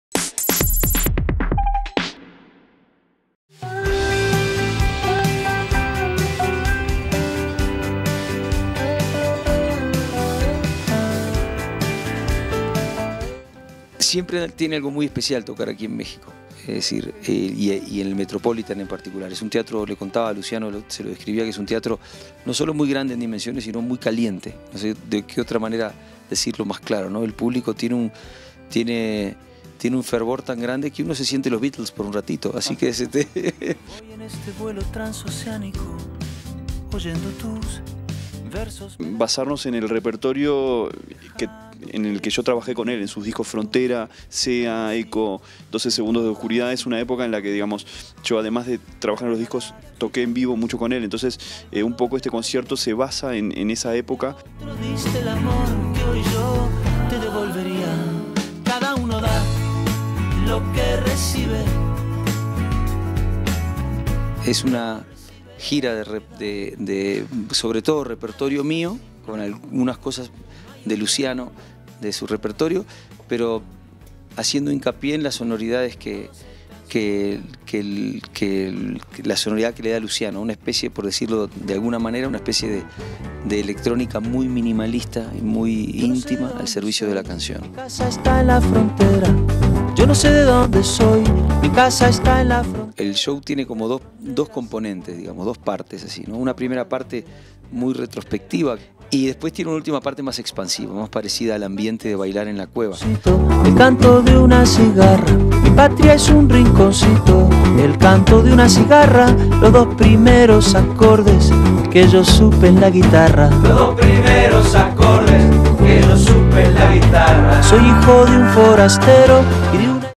Jorge Drexler y Luciano Supervielle al Metropólitan
Se reúnen nuevamente para presentar la gira “Perfume”